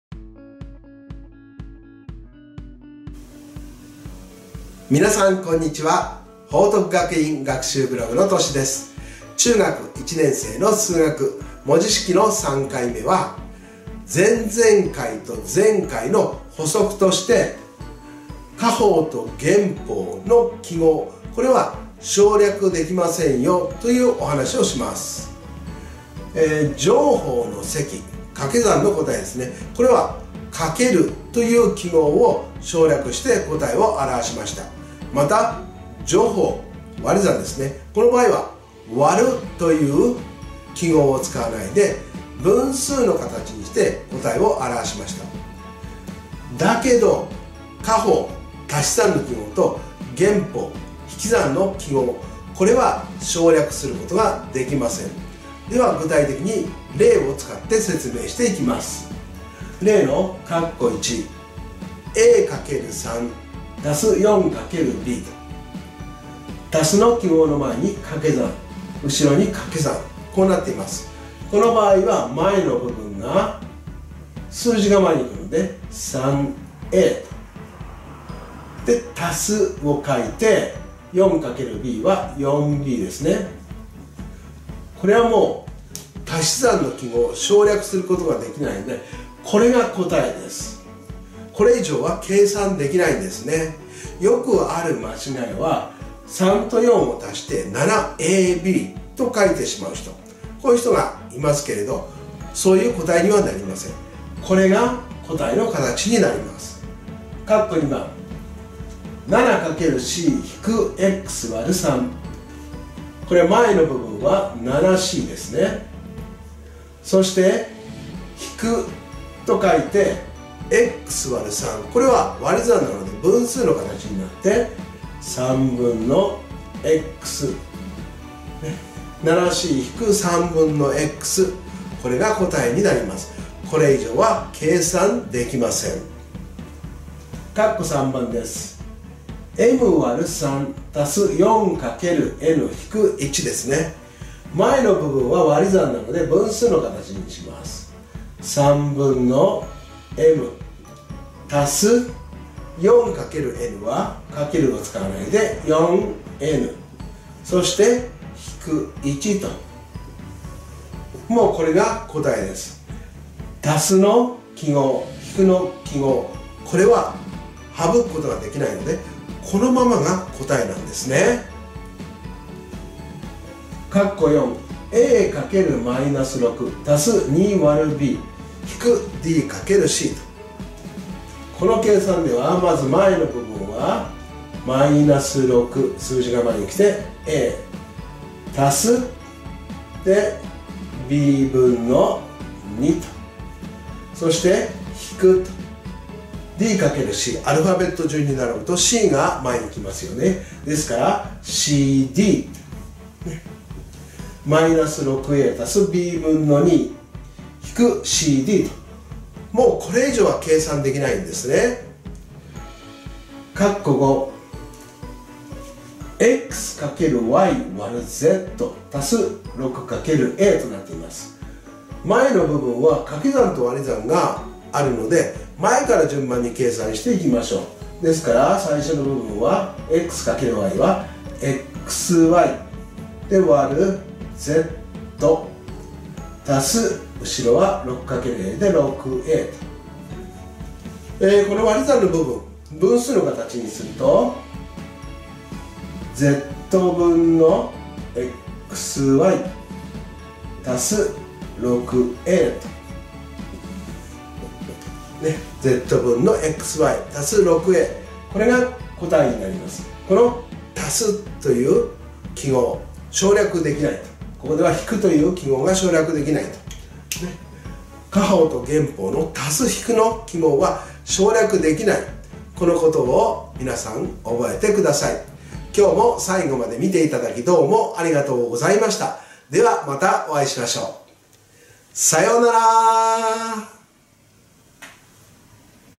加法と減法の記号は省略できない 中1数学 文字式３
文字式で、乗法の記号「×」は省略し、除法の記号「÷」は使わずに分数の形で商を表しますが、足し算と引き算の記号は省略することができませんよというお話をしました。